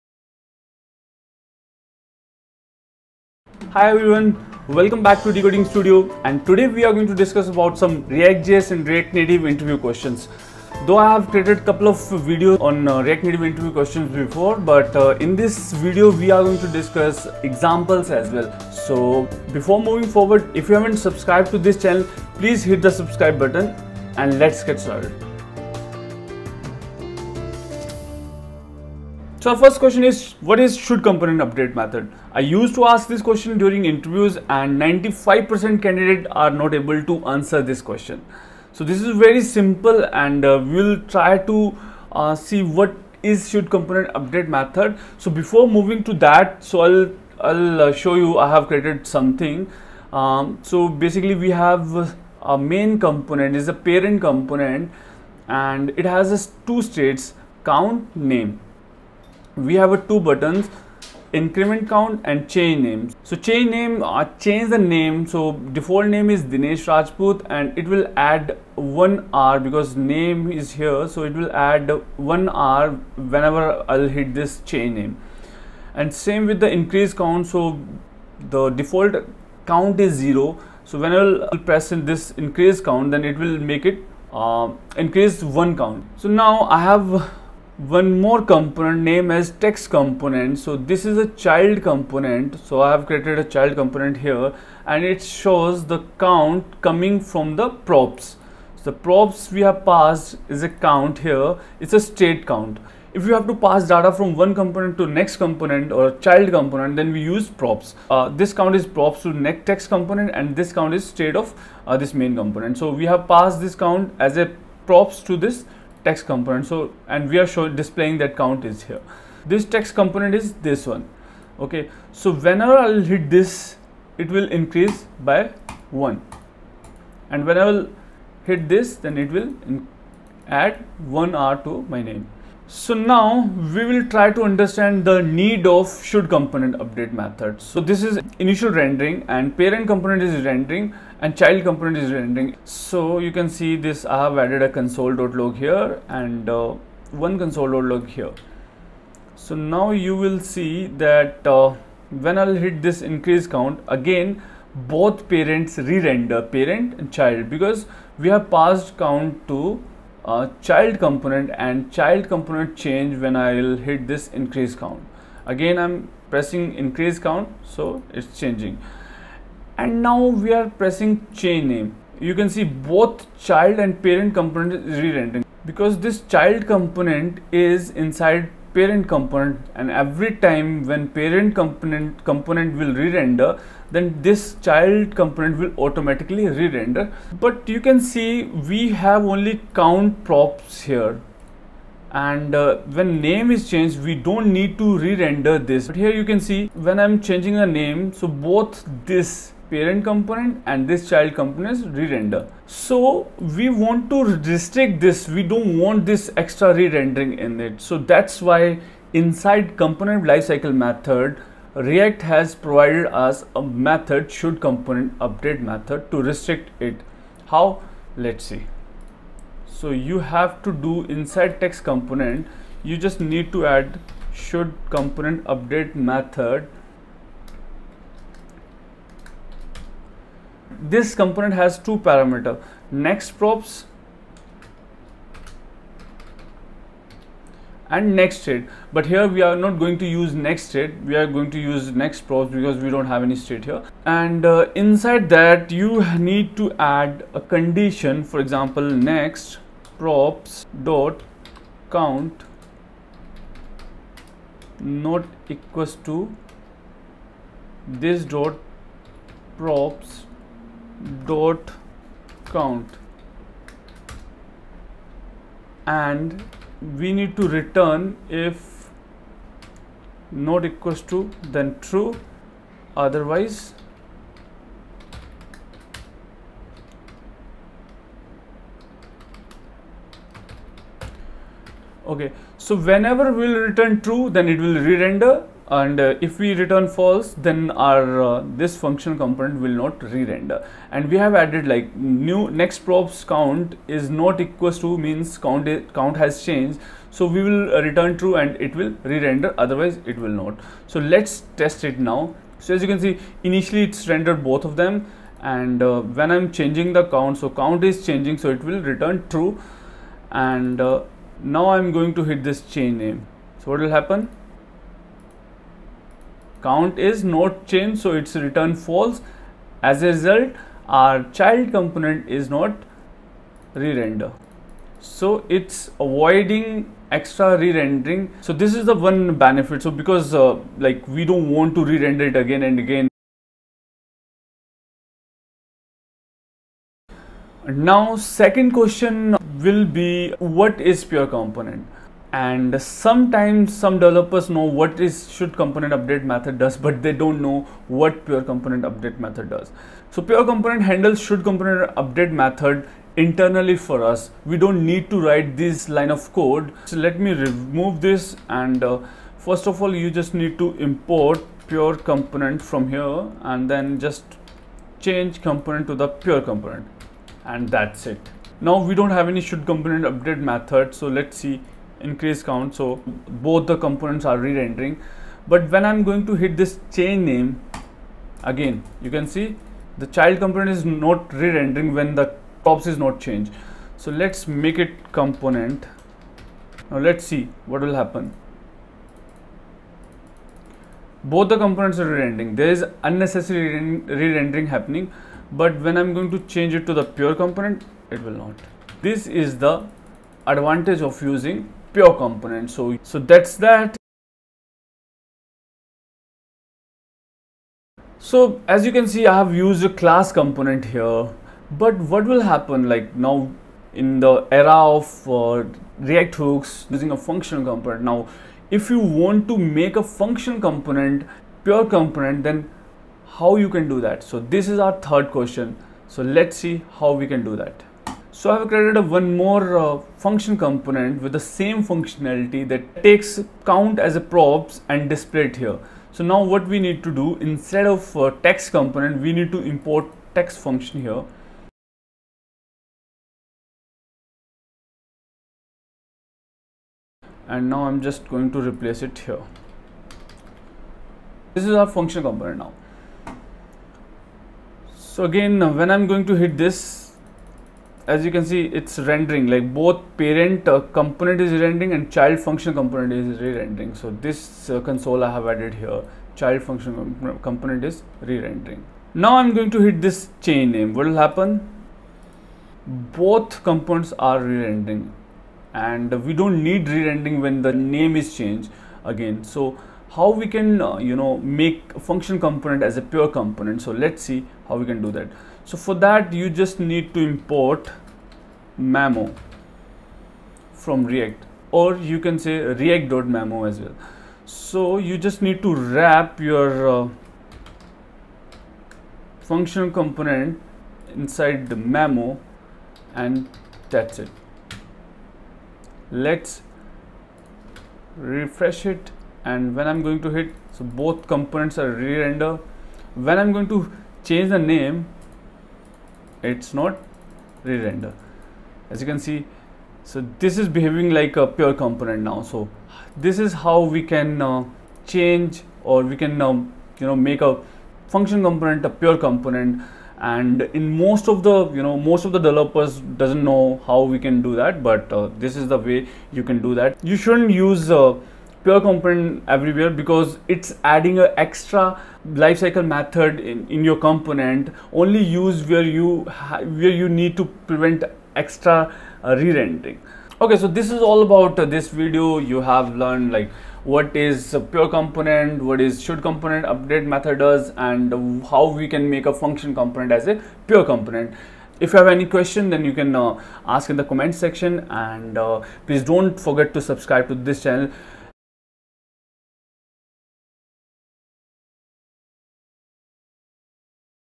React Native || ReactJS || Interview Questions with examples || Pure Component || memo || Part 3
In this video, we are going to learn a bunch of ReactJS and React Native interviews Questions/Answers with live examples.

I have also explained PureComponent, shoudComponentUpdate, and React.memo concepts in detail which are asked in interviews very frequently.